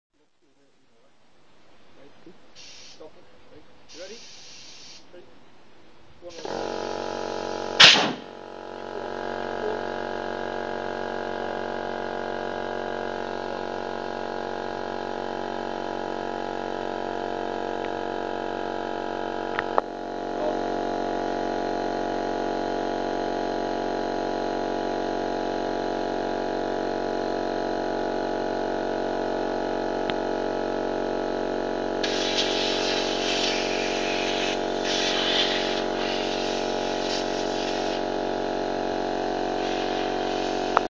Stretching a 195-45-15 admiral 712 onto a 9j banded steel wheel.
Just a quick vid of me and the youngin bead blasting a 195-45 onto a 15" banded 9j mazda rim.